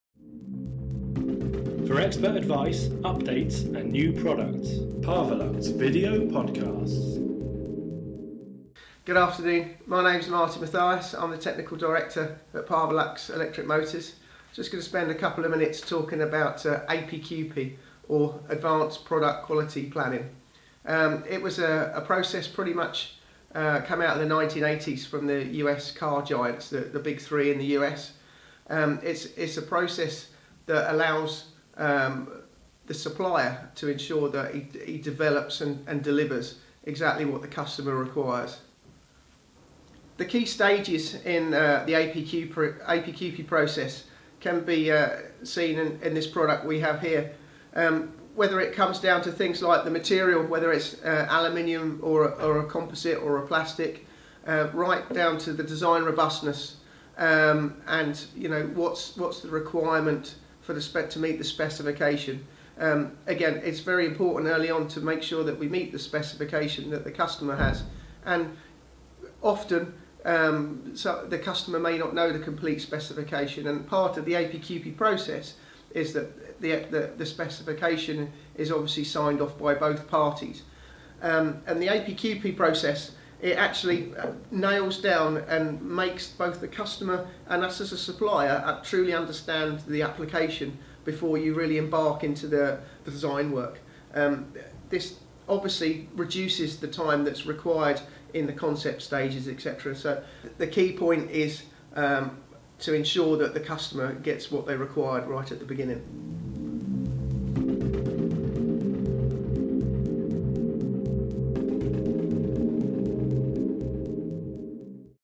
Parvalux Video Podcast 3.0: Advanced Product Quality Planning
Parvalux Technical Director Martin Mathias introduces Advanced Product Quality Planning (APQP). APQP is a process used by Parvalux to ensure that the design team develop and deliver exactly what an OEM requires by ensuring that each stage of the specification is signed-off.